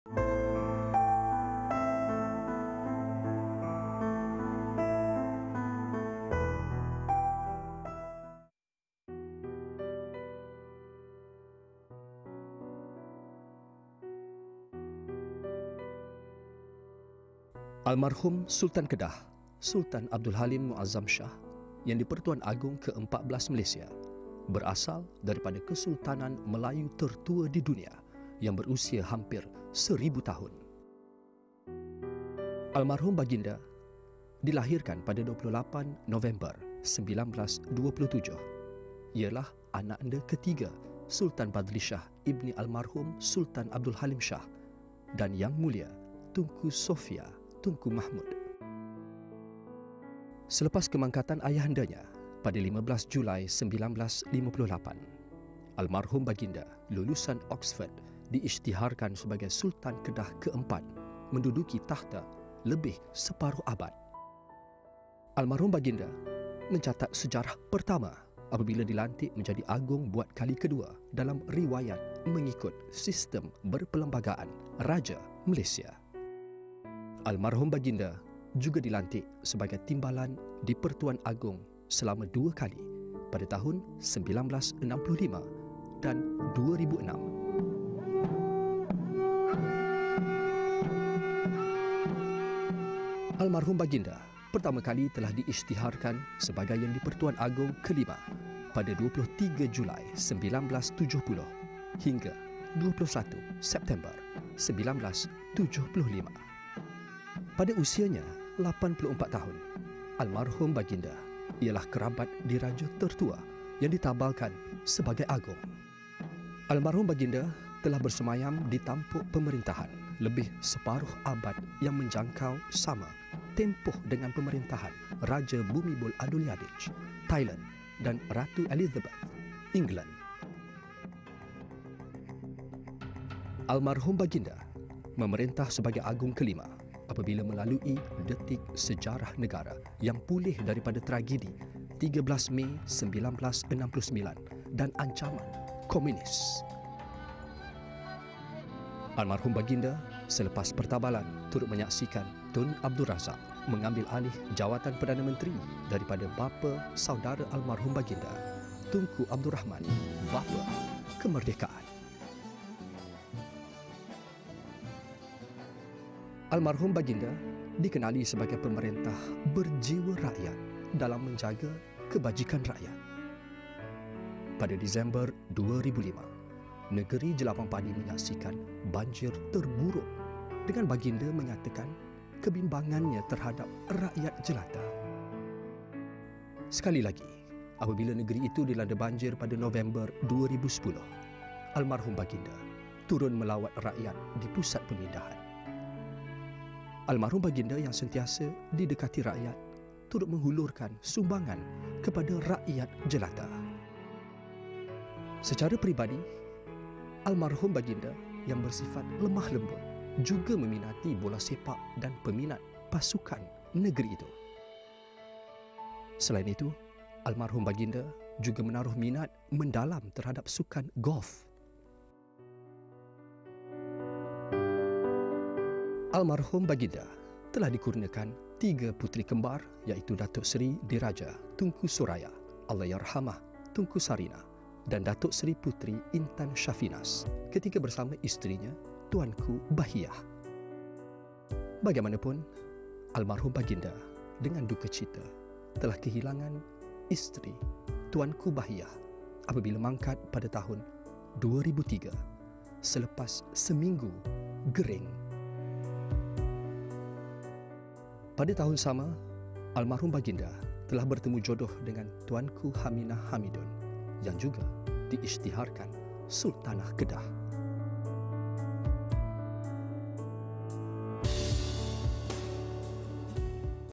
Sultan Kedah dalam kenangan
Almarhum Sultan Kedah, Sultan Abdul Halim Mu’adzam Shah, Yang Di-pertuan Agong ke-14 Malaysia, berasal daripada Kesultanan Melayu tertua di dunia yang berusia hampir seribu tahun.

Almarhum baginda yang dilahirkan pada 28 Nov 1927 ialah anakanda ketiga Sultan Badlishah Ibni Almarhum Sultan Abdul Hamid Halim Shah dan Yang Mulia Tunku Sofia Tunku Mahmud.